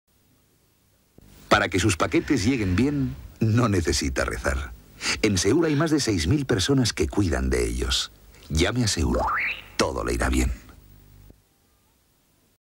Para que llegue su pedido no necesita rezar... (1993)- Anuncios SEUR
¿Rezar para que llegara un pedido? Con SEUR no era necesario. Por eso, si nos llamabas, todo te iba bien.

No te pierdas todos los anuncios en nuestra lista "Mensajeros del pasado: nuestra historia anuncio por anuncio"